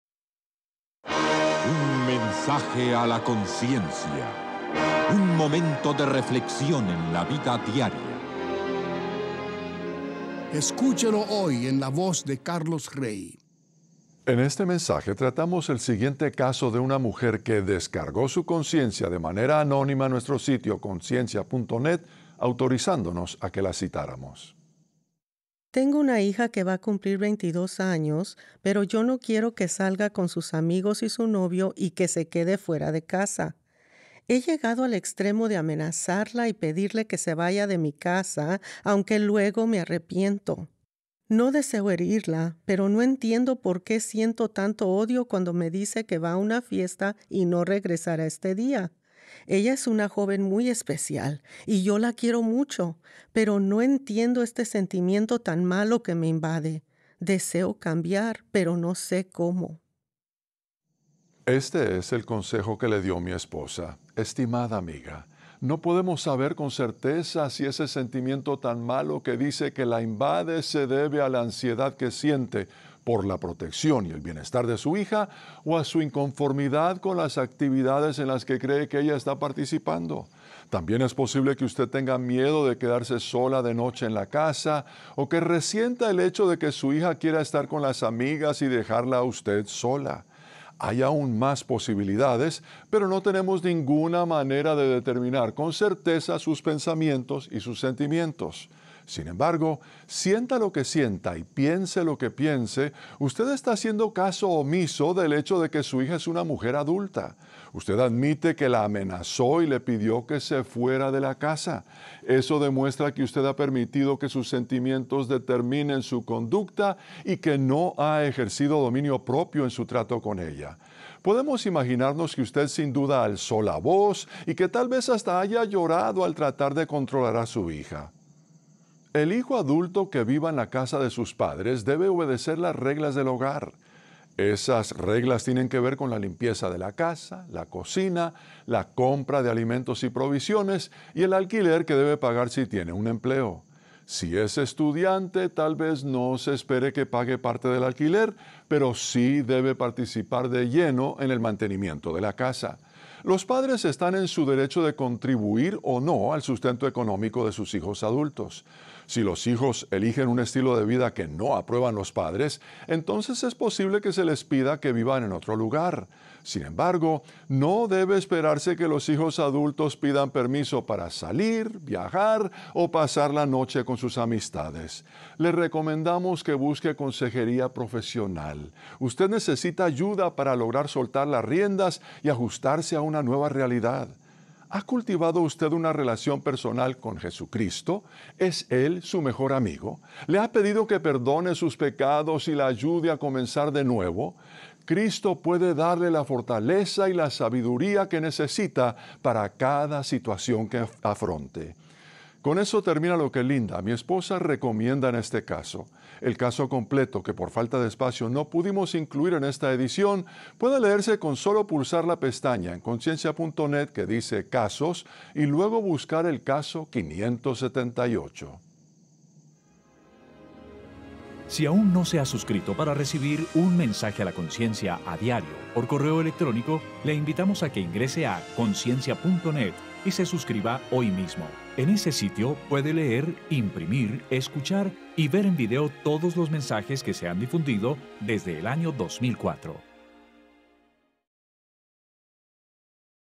«No entiendo por qué siento tanto odio»
«Tengo una hija que va a cumplir veintidós años, pero yo no quiero que salga con sus amigos y su novio y que se quede fuera de casa. He llegado al extremo de amenazarla y pedirle que se vaya de mi casa.»